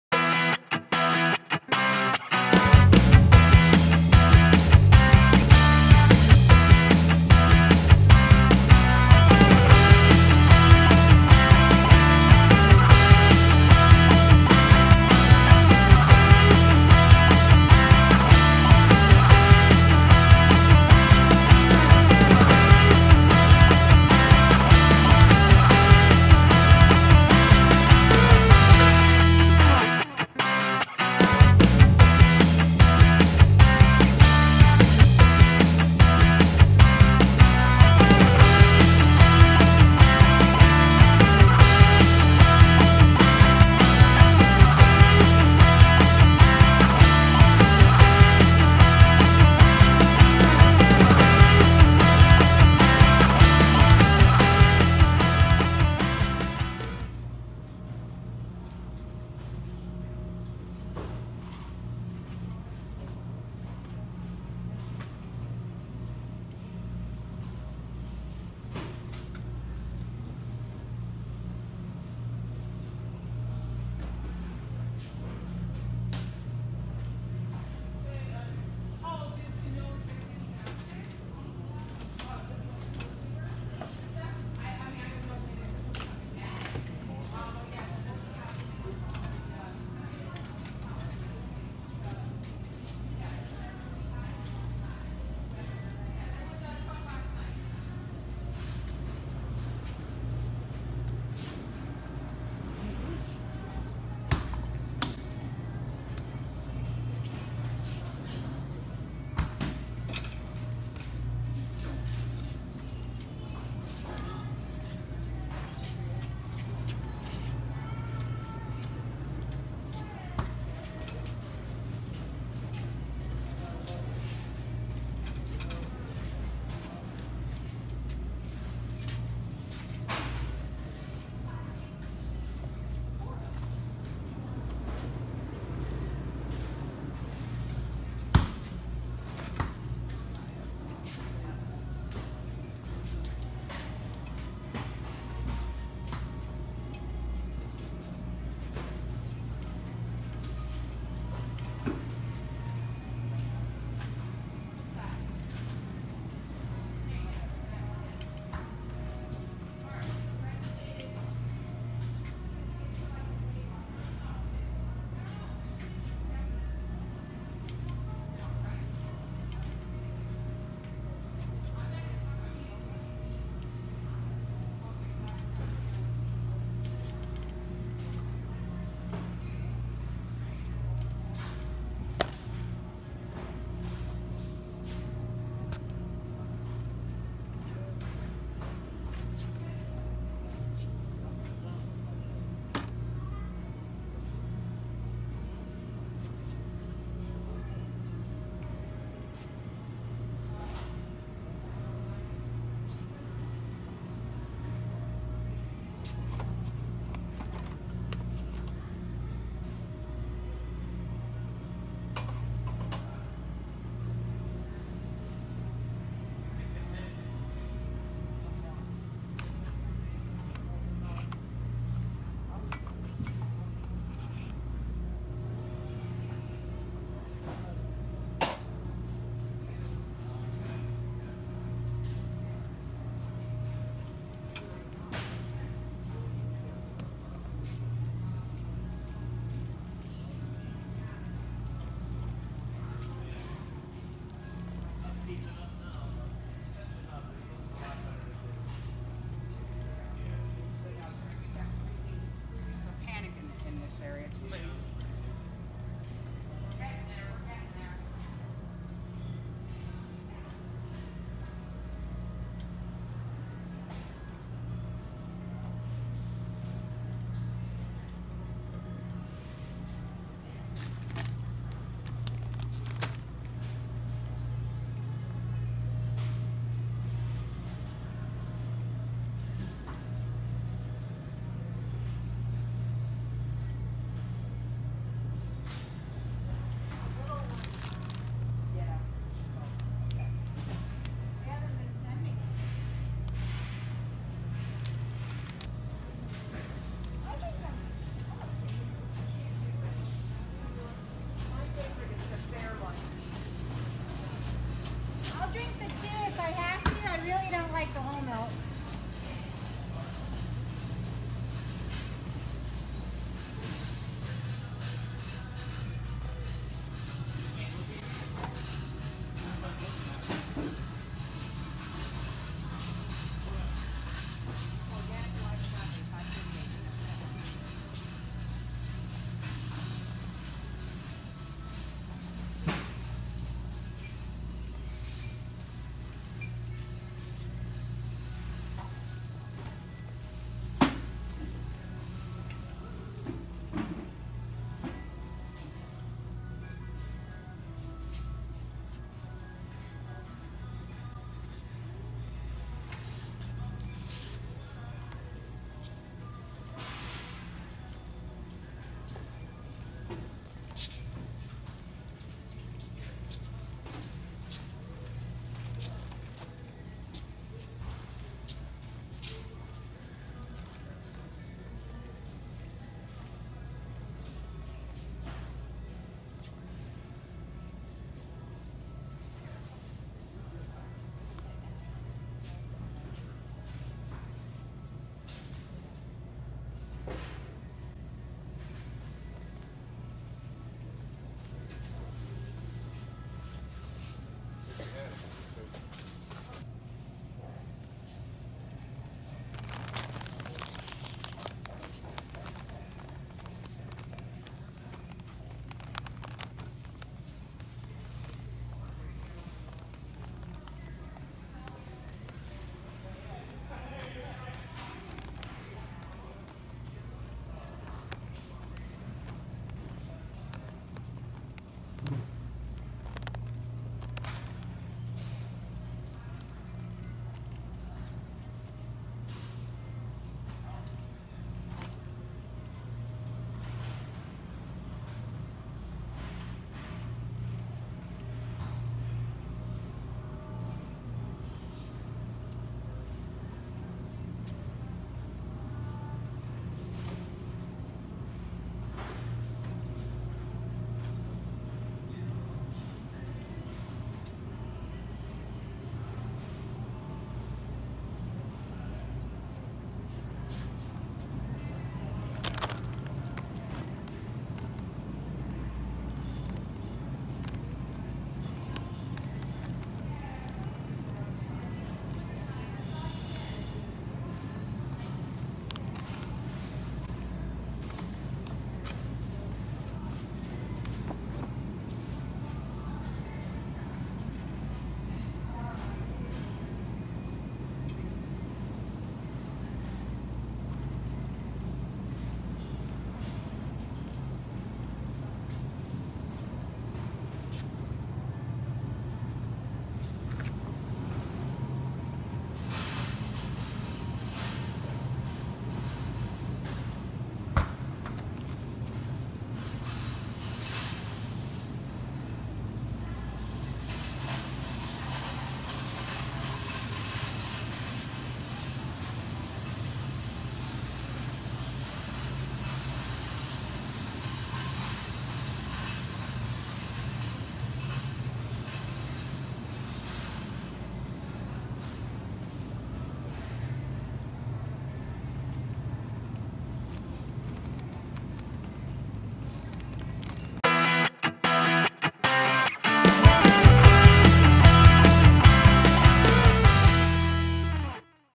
CORONAVIRUS SHOPPING/WAL MART SHOPPING IS SCARY/LESS IS BEST/ EPISODE 765/CHERYLS HOME COOKING
I went shopping to pick up a few things at the store. no music playing, hardly any shoppers. The store was some what stocked, they were out of a couple of items, but employees were restocking as we all shopped. Didnt see any people buying TONS of items. Forgot to even check if they had paper products. Very scary times we live in now. I wanted to share with you the Way Mart near me so you can see how the store is in my area.